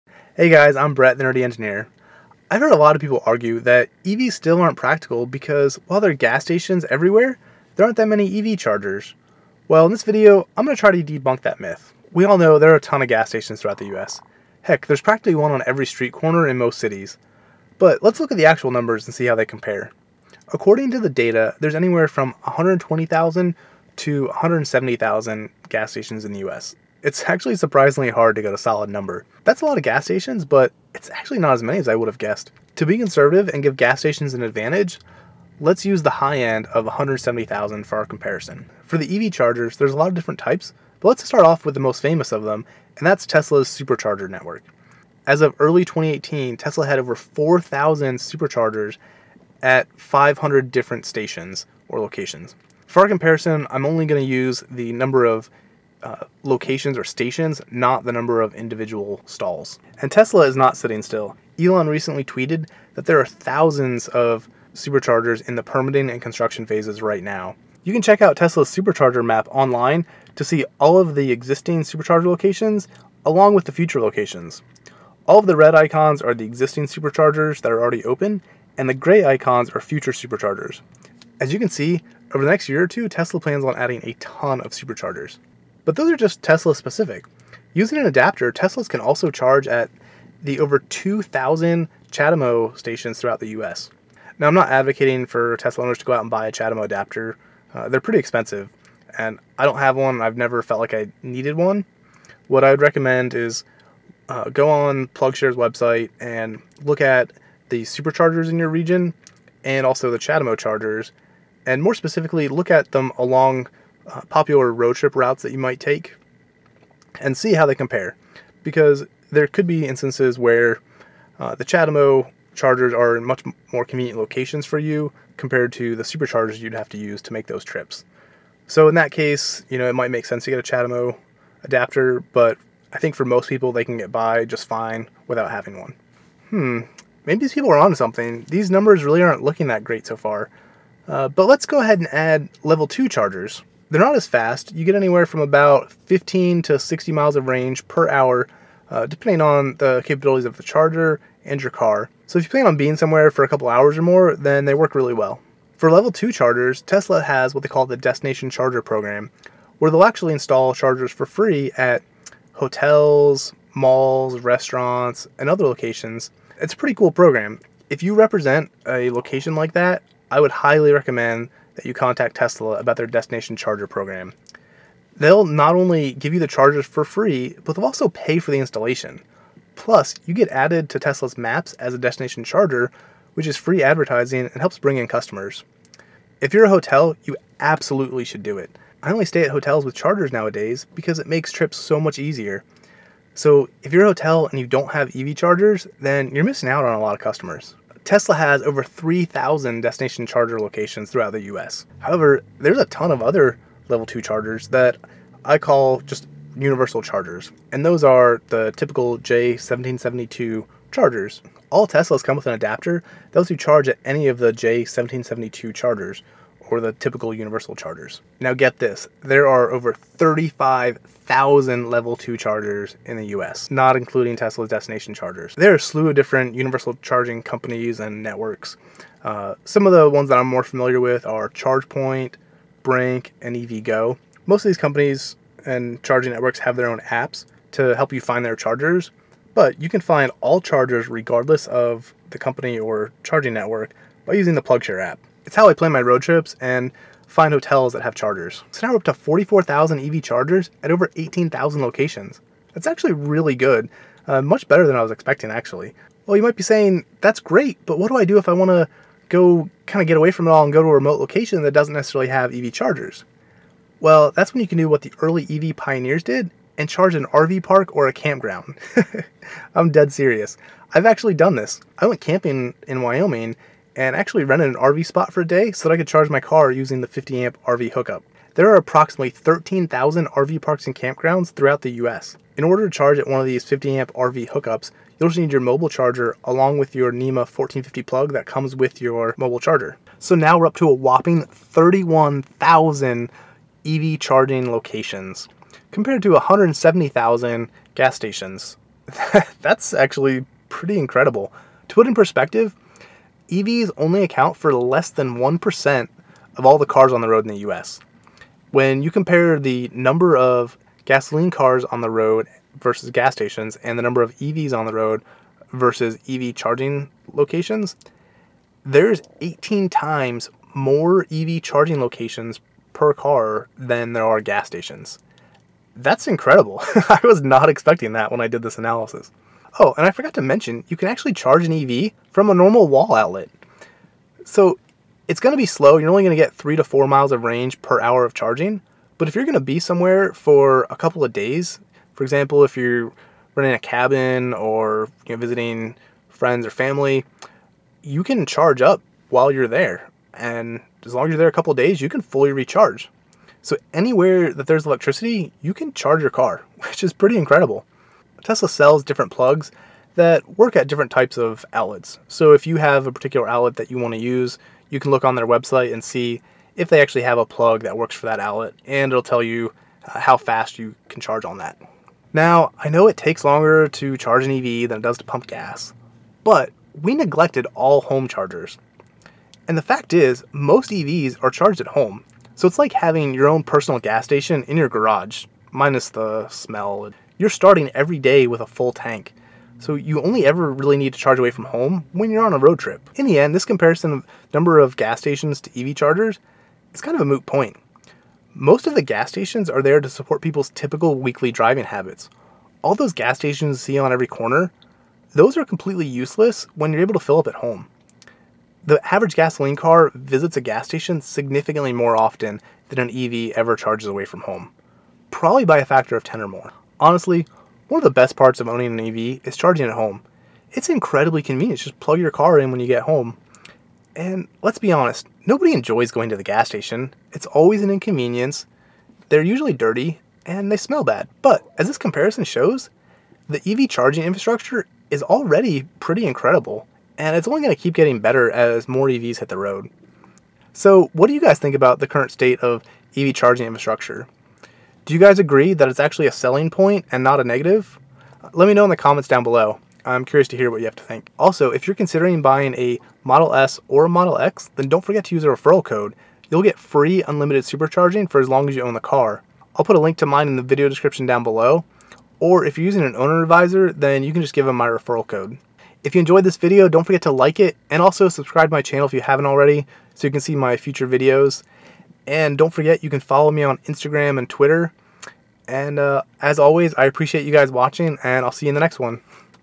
Gas Stations vs EV Chargers
Think EVs still aren't practical because there aren't as many chargers as gas stations? Think again!

My Tesla Gear: